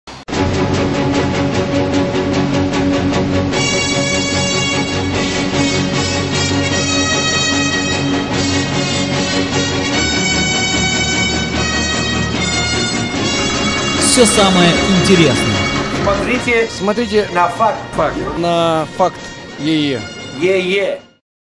Рекламный ролик.avi
Рекламный ролик для информационно развлекательного портала  FACT.ee с участием Dj Bonebreaker, Юрия Шевчука и Александра Чернецкого